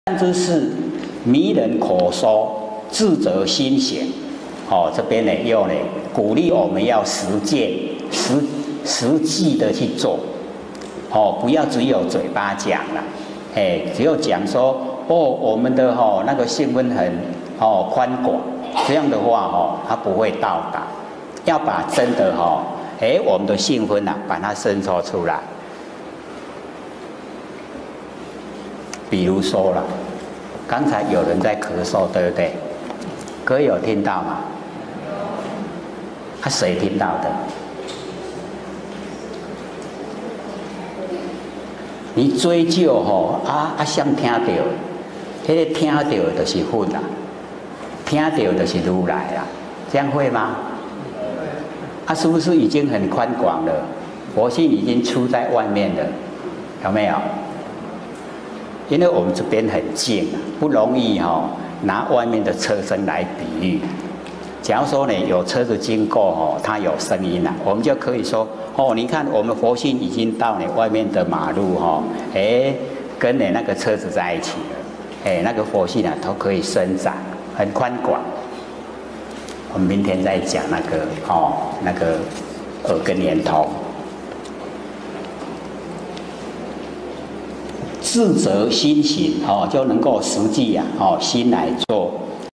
把性分伸展出來|誰聽到?
「善知識，迷人口說，智者心行」
   這邊又鼓勵我們要實踐，實際地去做，不要只有嘴巴講啦，只有講說「喔…我們的性分很寬廣」，這樣的話祂不會到達，要真的把我們的性分把祂伸展出來，比如說剛才有人在咳嗽，對不對？各位有聽到嗎？誰聽到的？你追究誰聽到的？那個聽到的就是佛，聽到的就是如來，這樣會嗎？是不是已經很寬廣了？佛性已經出在外面了，有没有？因為我們這邊很靜，不容易拿外面的車聲比喻。假如有車子經過有聲音，我們就可以說我們的佛性已經到外面的馬路跟車子在一起，那個佛性都可以伸展，很寬廣，我們改天再講耳根圓通。智者心行，就能夠實踐心來做。